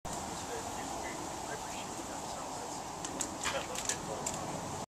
repaired break in sewer pipe by florida plumber (tampa).MP4
Swift Plumbers of Florida , repaired break in main drain by clean out